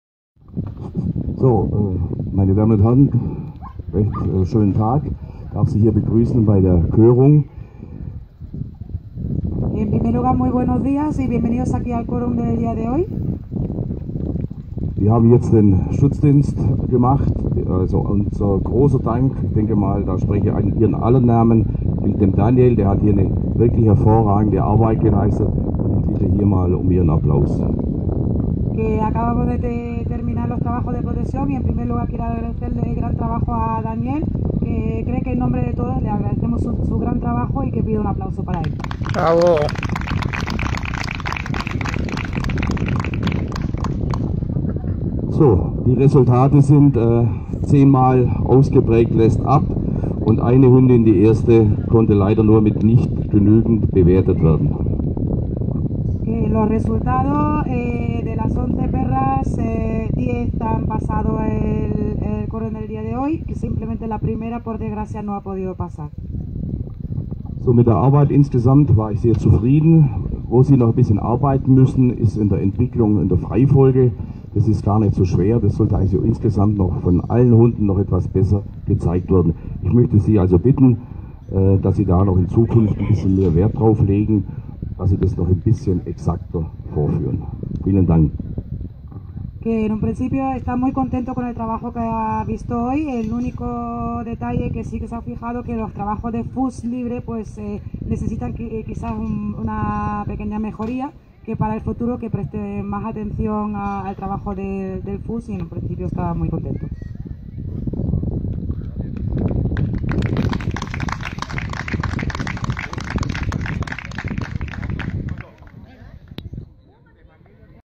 Monografica RCeppa Sierra Nevada 2015
Felicitaciones del Juez Rainer Mast al figurante de la Monografica Daniel Bensusan Duque